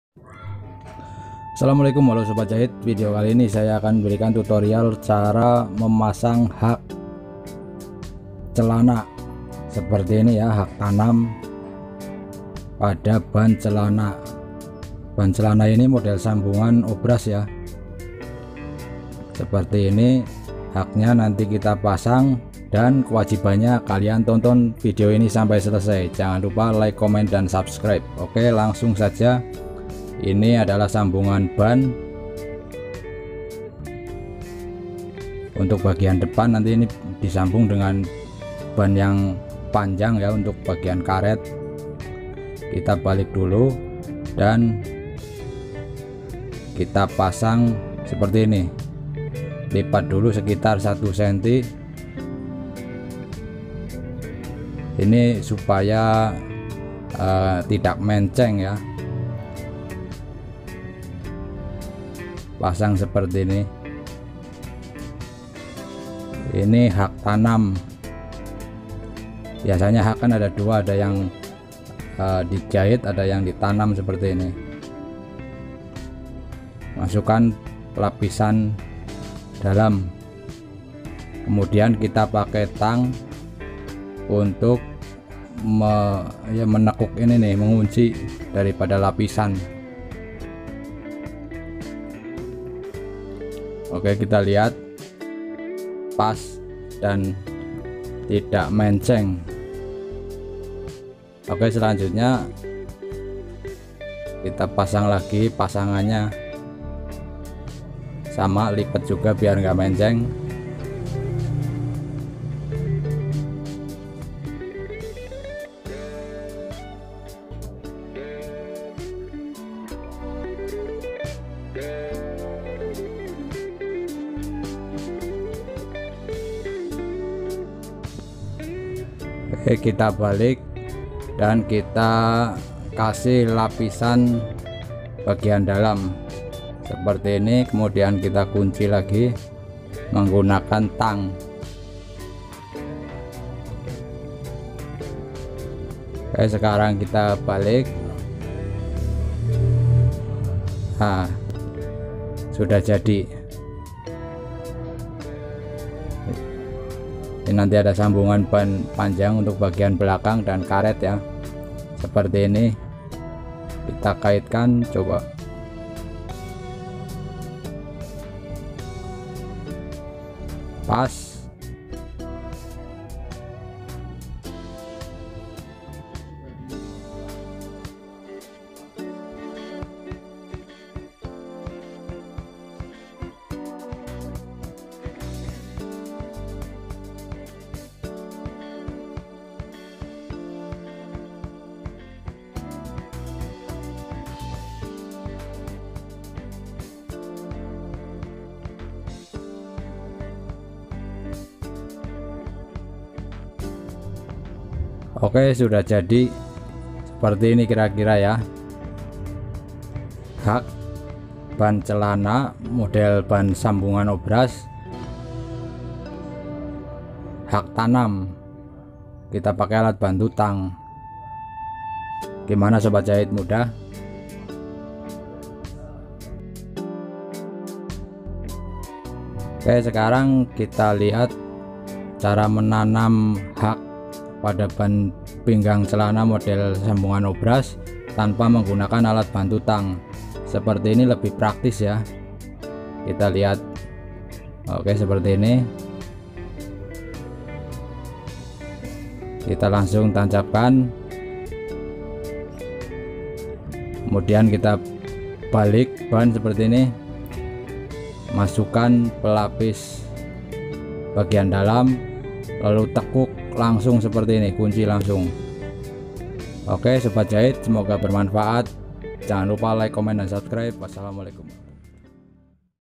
Cara Memasang Hak Celana Dengan Cepat
Cara memasang hak celana dengan cepat dan rapi,semoga bermanfaat